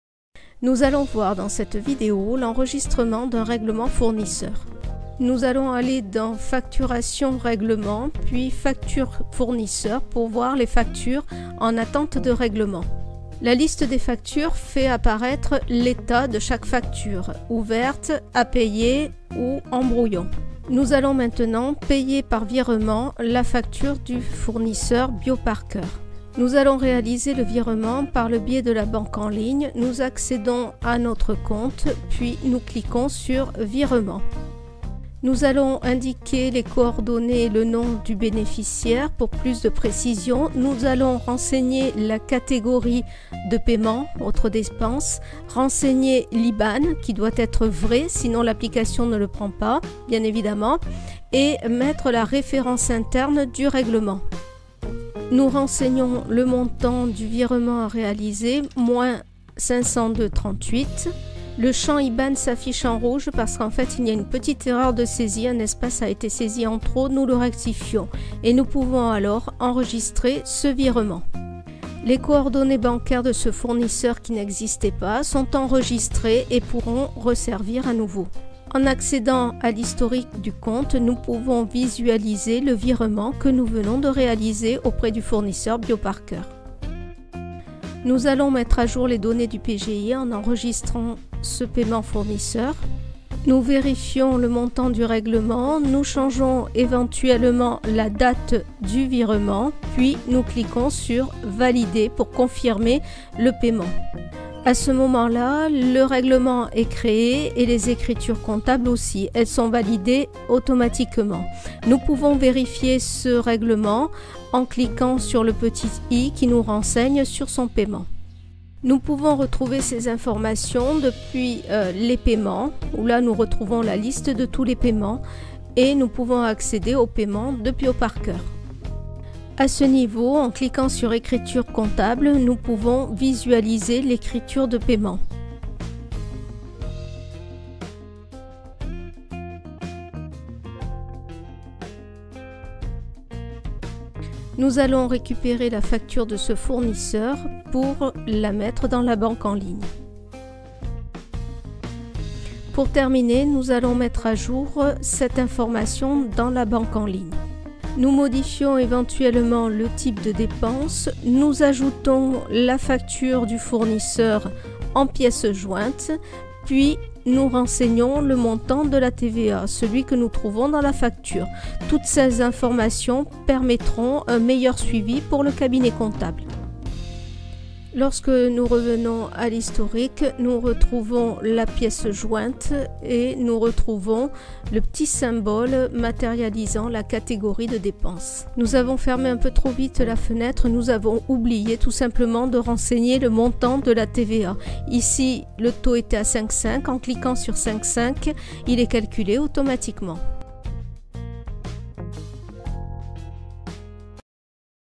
OdooV11 - L'enregistrement d'un règlement fournisseur 1reglement-simple-virement
Cette vidéo présente l'enregistrement dans le PGI Odoo du règlement d'une facture fournisseur. La facture fournisseur est payée par virement bancaire sur la banque en ligne (bénéficiaire, date, montant, libellé du virement). Le bénéficiaire du virement est créé sur la banque en ligne (nom - IBAN). Une fois le virement réalisé sur la banque en ligne, il apparait dans l'historique des mouvements. Cette BqEnLigne permet d'identifier le type de dépense et de joindre la facture pdf. Ceci est très utile pour le suivi comptable du cabinet comptable. Dans le PGI, le paiement se fait depuis la facture fournisseur. L'écriture comptable est générée automatiquement tout comme le lettrage du compte de tiers.